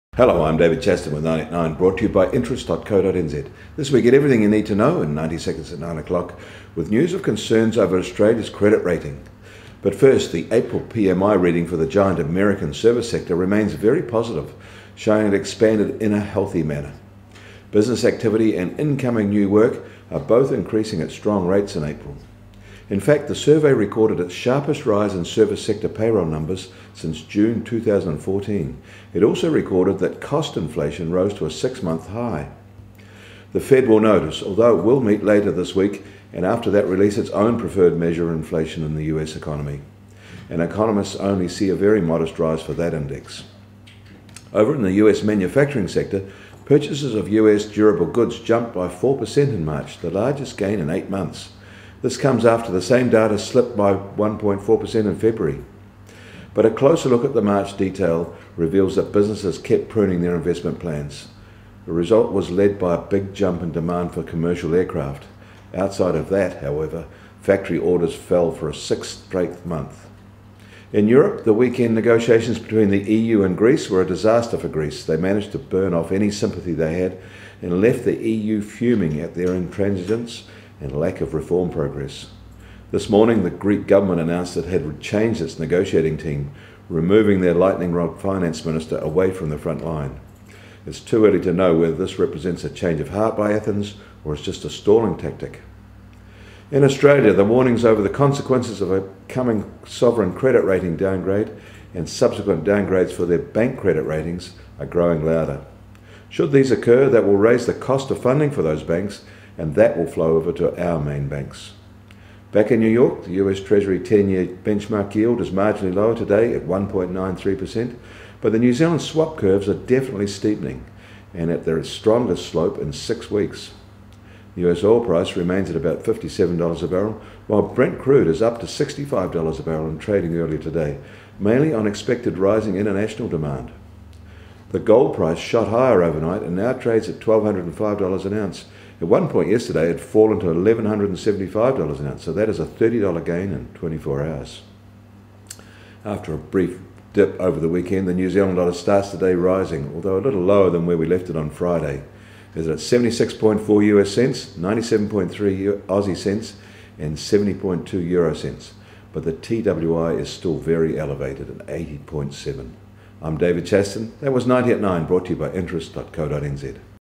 90 seconds @ 9am: Australia warned on ratings
The April PMI reading for the giant American service sector remains very positive showing it expanding in a healthy manner. Business activity and incoming new work are both increasing at strong rates in April. In fact, the survey recorded the sharpest rise in service sector payroll numbers since June 2014. It also recorded that cost inflation rose to a six-month high.

The Fed will notice, although it will meet later in the week, and after that release its own preferred measure of inflation in the USA economy. And economists only see a very modest rise for that index.

Over in the US manufacturing sector, purchases of US durable goods jumped by +4% in March, the largest gain in eight months. This comes after this same data slipped by -1.4% in February. But a closer look at the March detail reveals that businesses kept pruning their investment plans. The result was led by a big jump in demand for commercial aircraft. Outside of that however, factory orders fell for a sixth straight month.